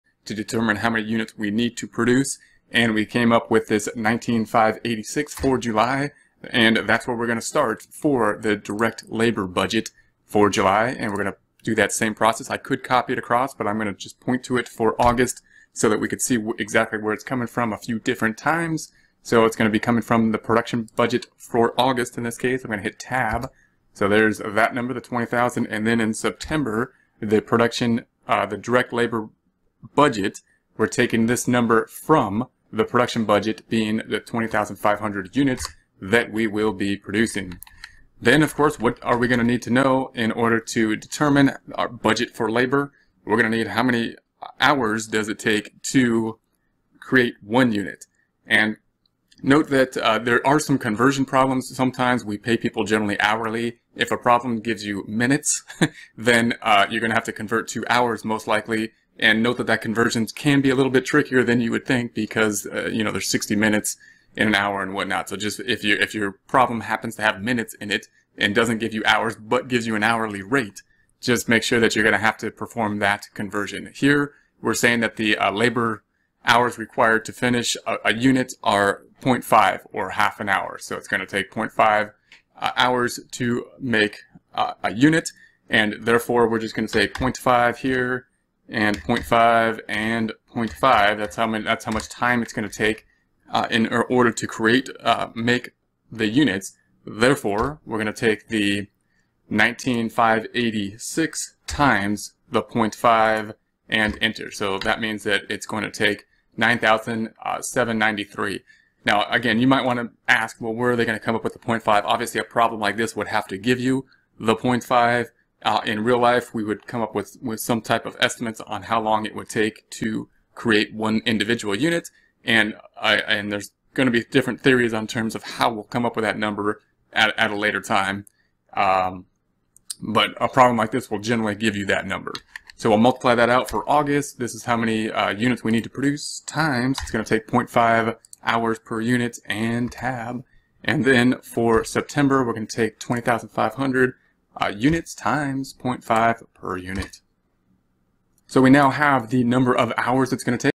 Calculating Labor Requirements Master Budgets
In this video, we focus on calculating labor requirements within master budgets. Learn how to determine production units needed, calculate labor hours per unit, and estimate labor costs. This tutorial is perfect for accounting students and professionals aiming to enhance their budgeting skills and efficiency.